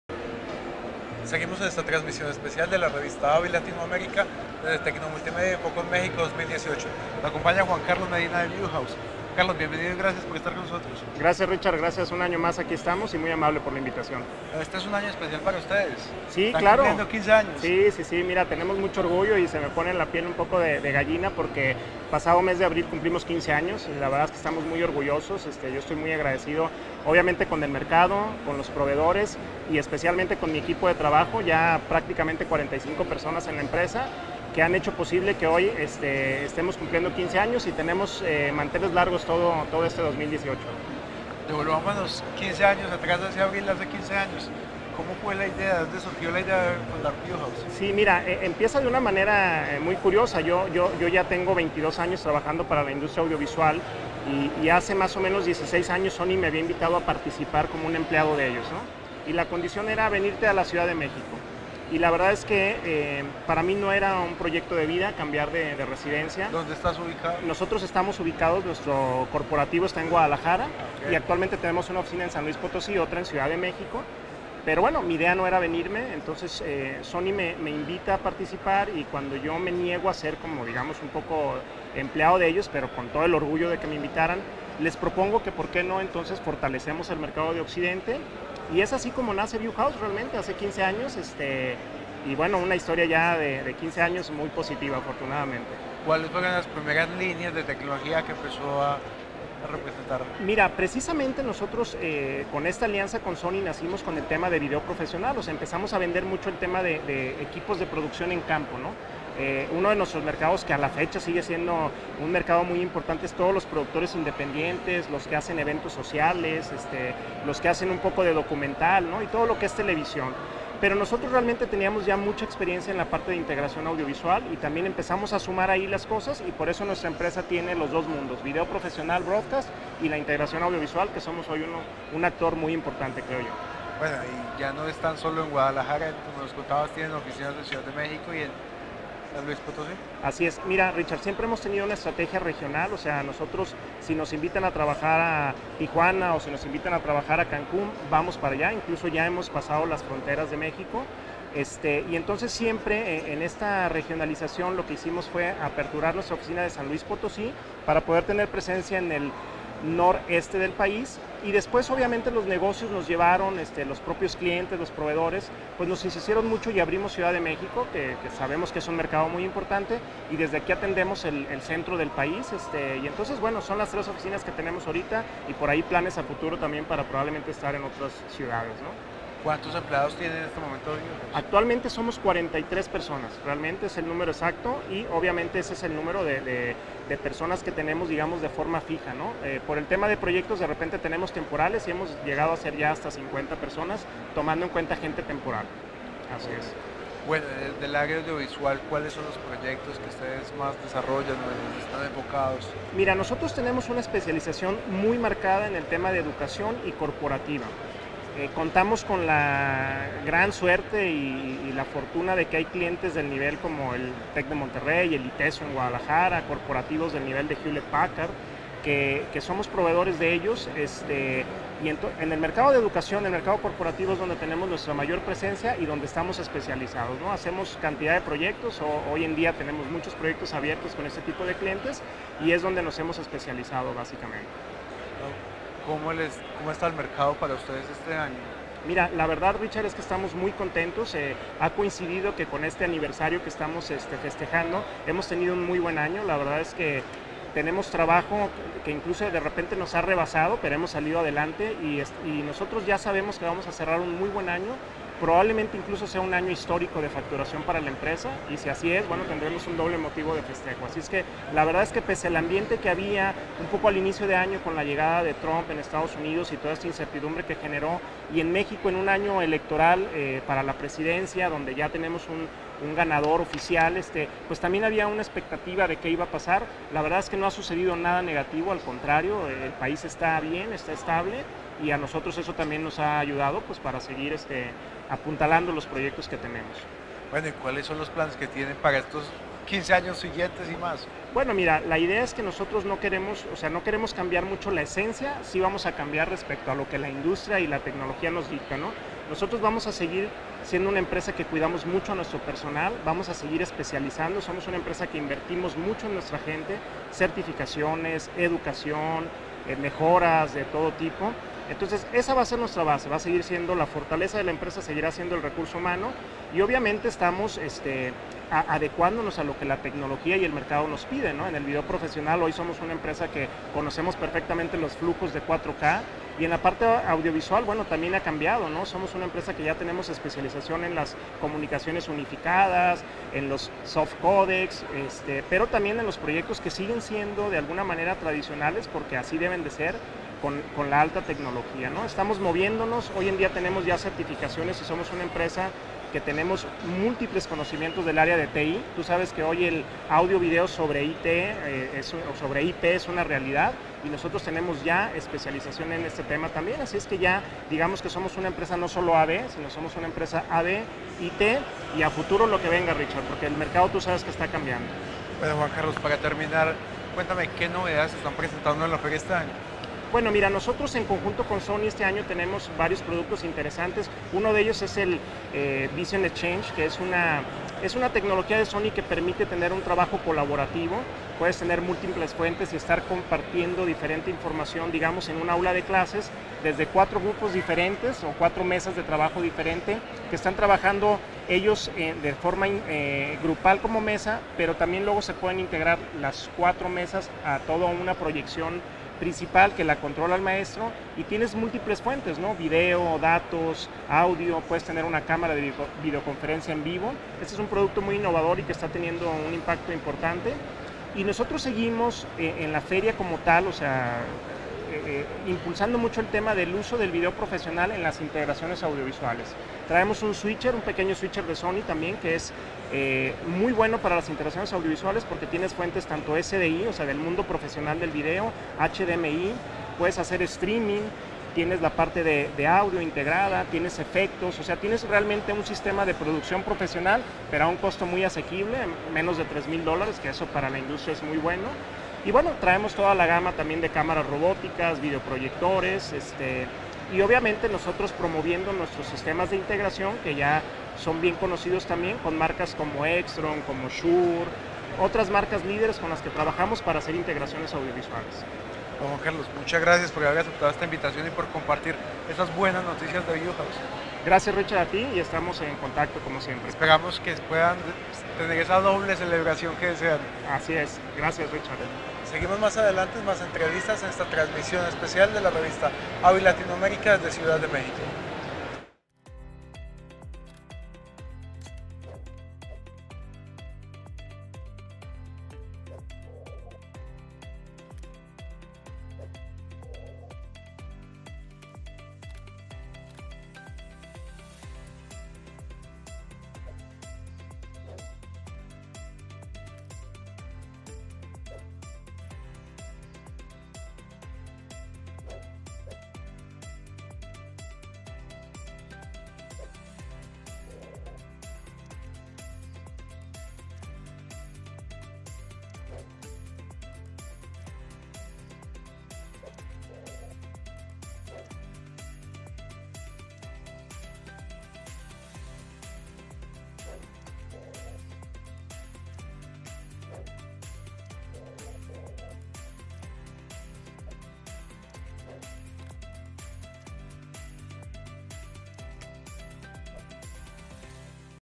Entrevista: Juan Carlos Medina de Viewhaus
Entrevista con Juan Carlos Medina de Viewhaus desde TecnoMultimedia InfoComm México 2018